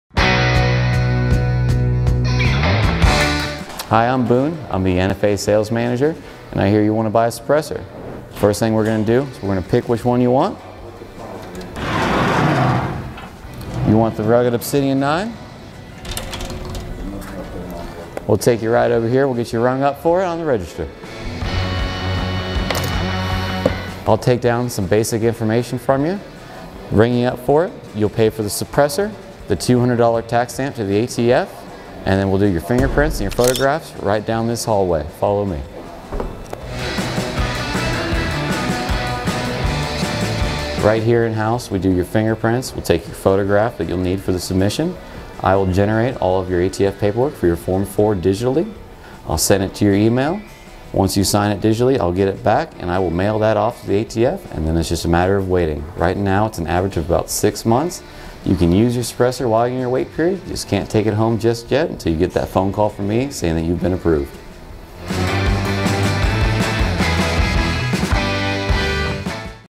Emerald Coast Indoor Shooting & Sport - How to Buy a Suppressor
The nfa sales manager from Emerald Coast Indoor Shooting & Sport telling the process of buying a suppressor.